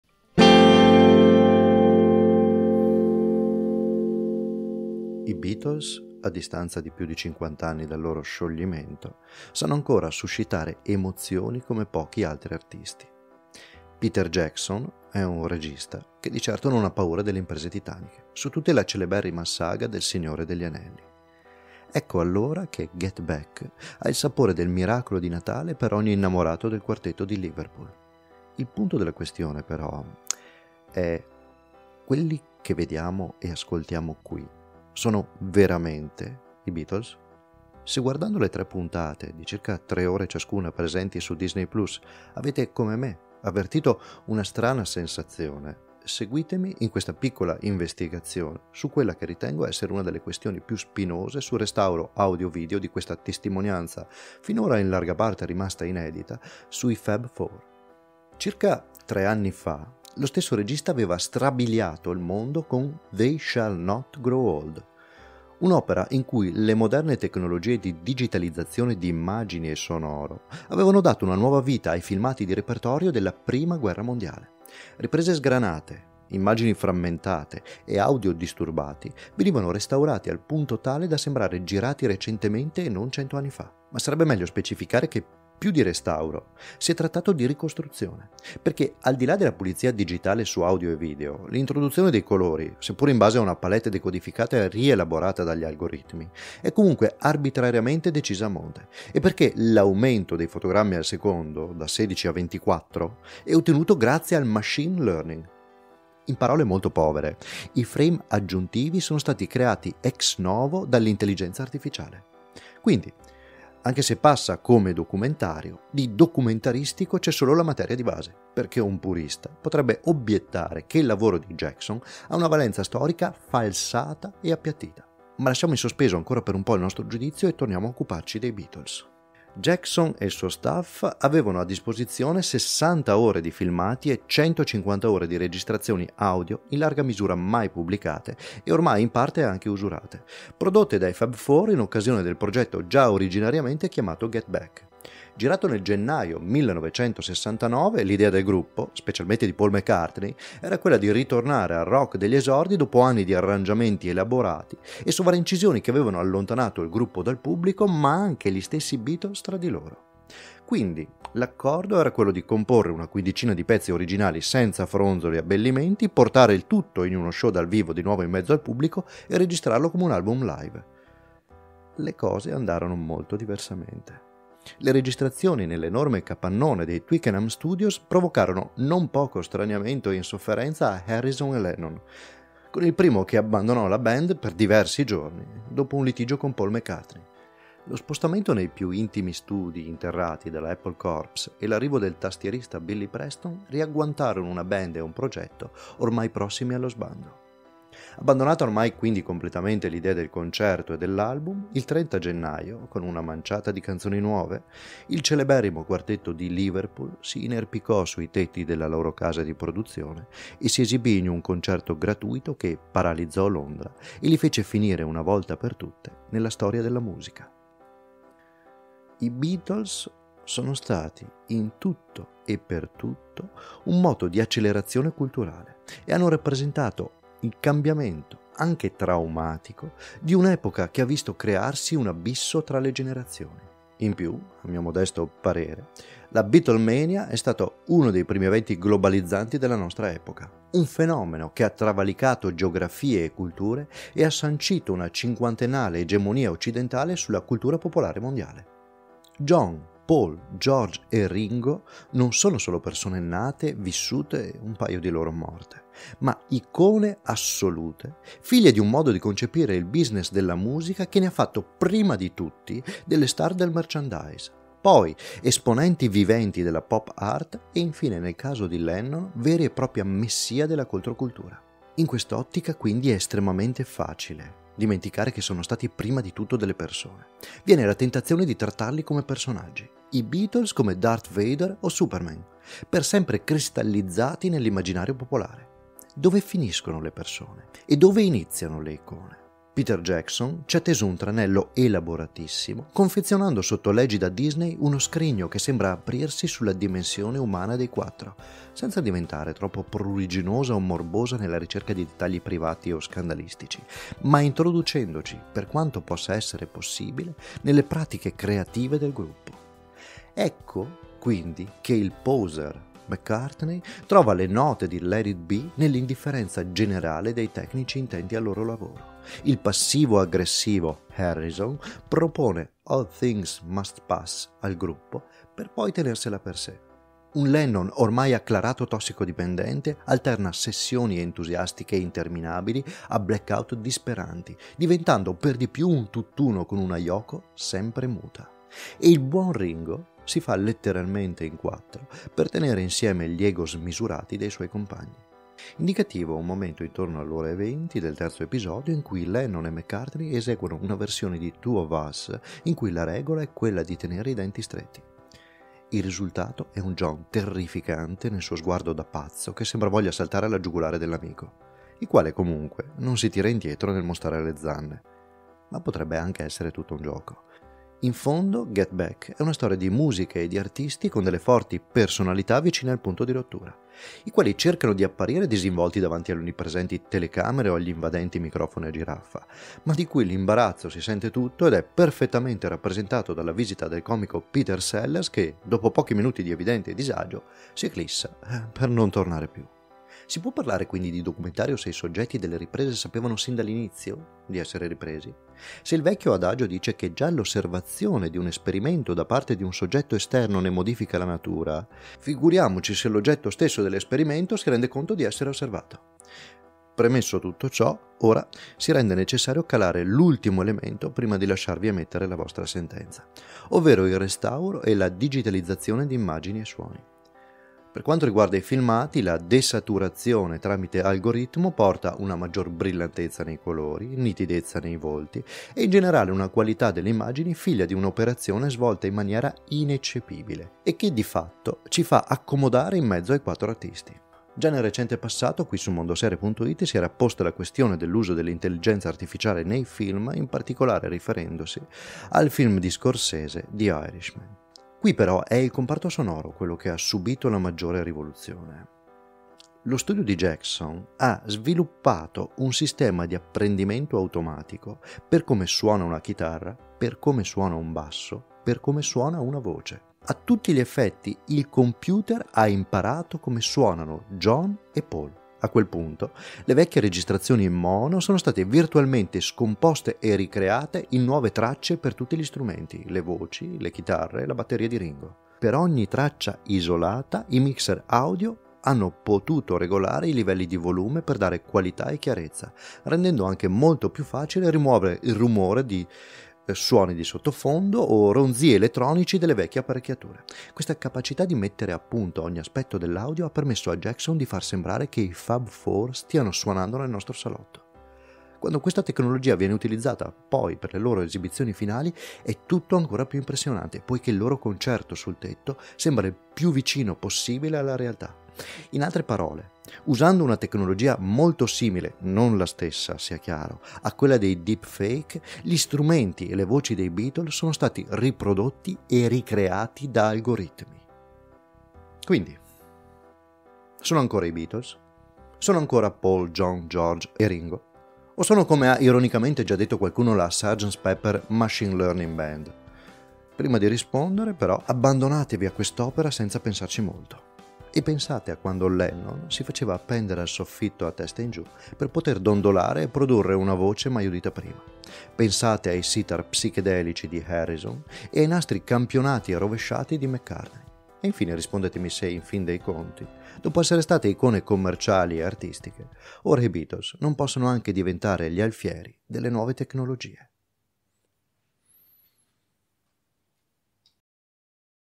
Get back riporta i Beatles in vita… o no? | PODCAST
Puntata a cura di Untimoteo.
Con Get Back, il regista de “Il Signore degli Anelli” Peter Jackson realizza un’opera emozionante e straniante, e non solo per i fanatici dei Beatles. Condensando una mole mostruosa di materiale in 3 parti per 8 ore (su Disney+). Facendoci vivere in prima persona le quattro settimane di inizio 1969 che culminarono nel leggendario concerto sul tetto della Apple Corps. Ma anche costringendoci a riflettere sull’uso dell’intelligenza artificiale nell’arte. E sul labile confine tra la dimensione umana e quella iconica della più grande pop band di tutti i tempi.
"Documentari" è il format del podcast di Mondoserie dedicato all'approfondimento delle produzioni non di fiction.